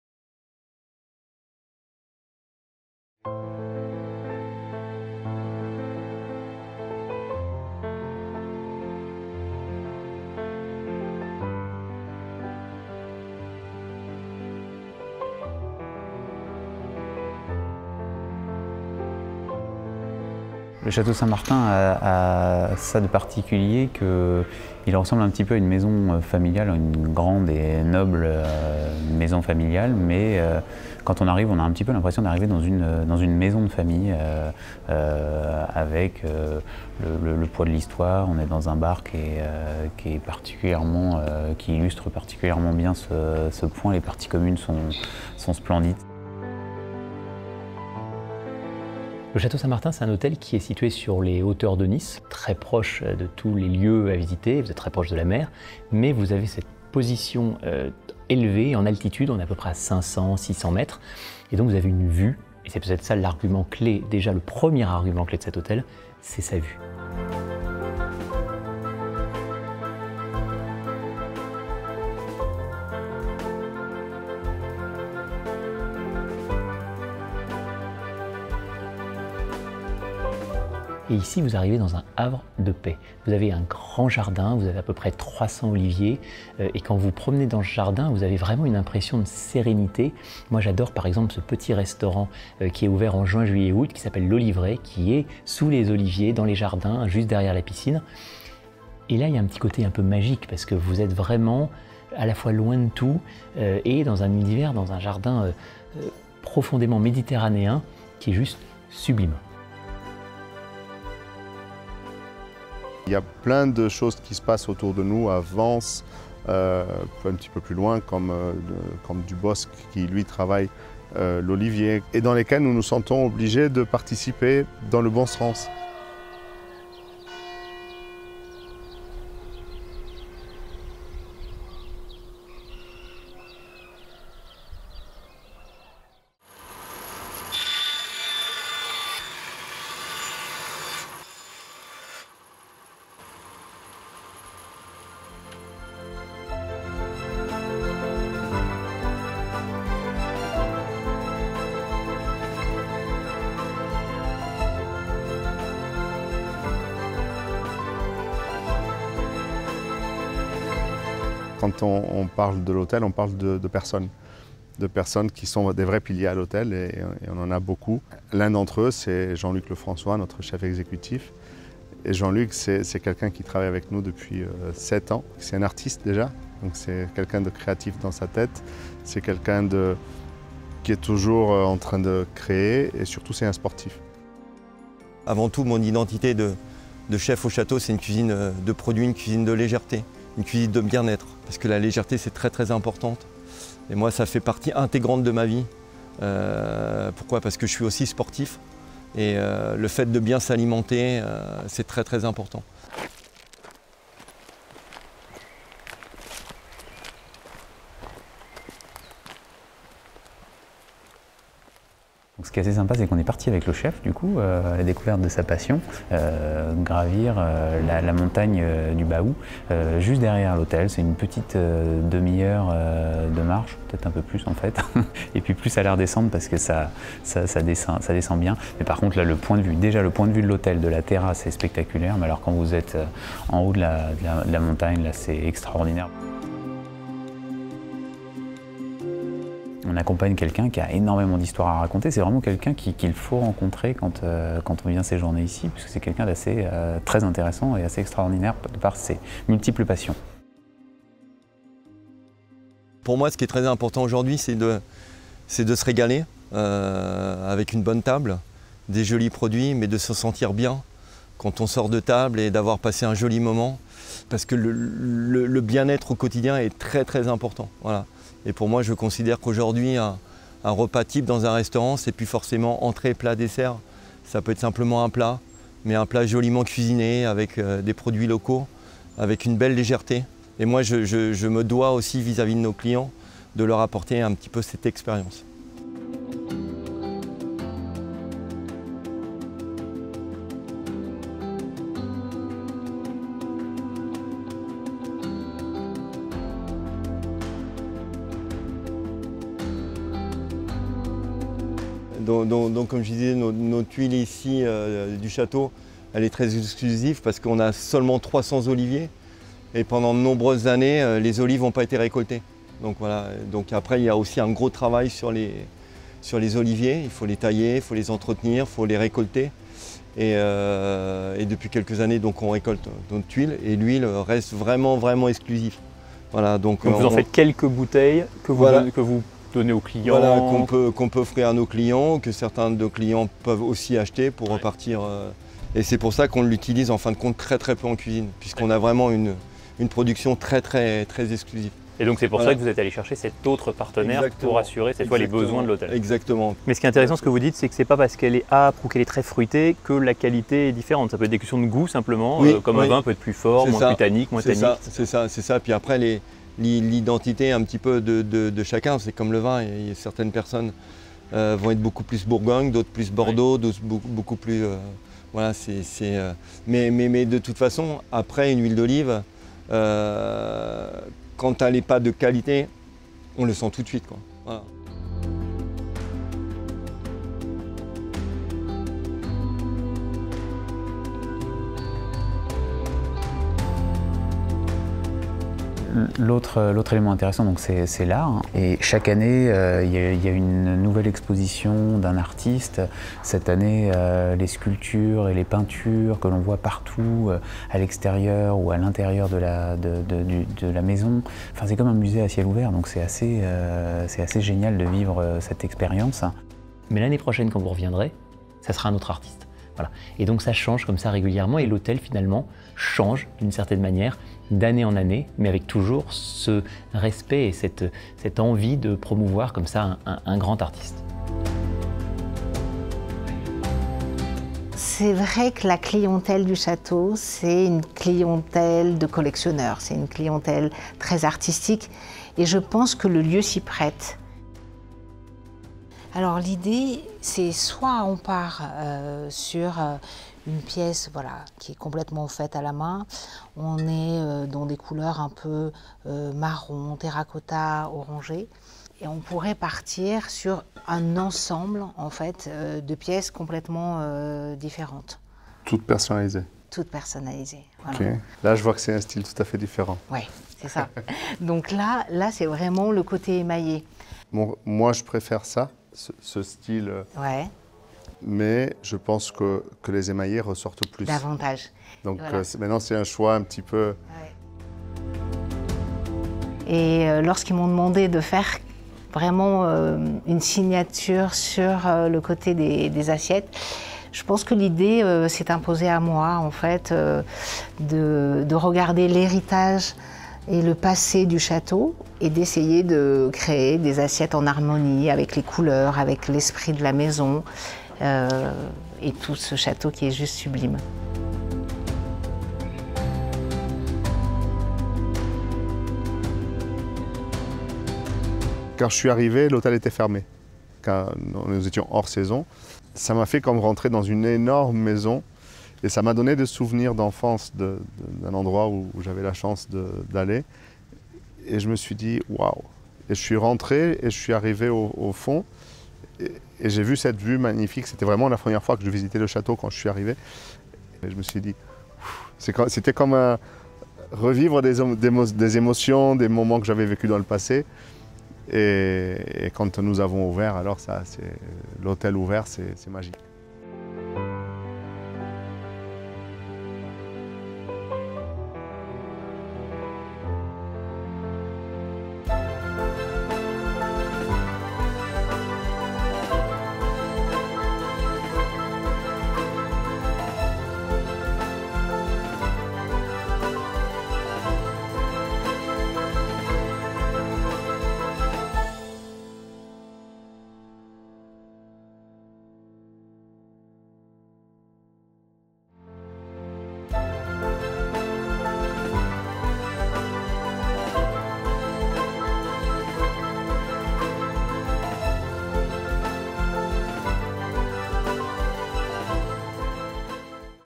Château Saint-Martin & Spa, a getaway to the French Riviera.
It’s more than just a pretty hotel on the French Riviera; It’s a Provencal retreat, hidden amid olive groves and gardens, and with interiors adorned in artworks. Château Saint-Martin is a different experience of the French Riviera...one that’s quieter and more exclusive, like staying at the home of a noble family.

This week, discover a historic estate that was once a Knights Templar castle, in the company of of General Manager Duarte Bon de Sousa, executive chef Jean-Luc Lefrançois and artist Agnès Sandahl, and of course, co-founders of Grand Luxury Hotels, brothers Ivan & Rouslan Lartisien!

00:00 Introduction with Rouslan Lartisien and Ivan Lartisien, co-founders of Grand Luxury Hotels
01:54 Integration of the local community with Duarte Bon de Sousa, General Manager at Chateau Saint-Martin & Spa
02:40 Chef Jean-Luc Lefrançois at Chateau Saint-Martin & Spa
06:25 Art in the hotel
07:08 Meeting with Agnès Sandahl
08:28 Conclusion with Duarte Bon de Sousa, General Manager at Chateau Saint-Martin & Spa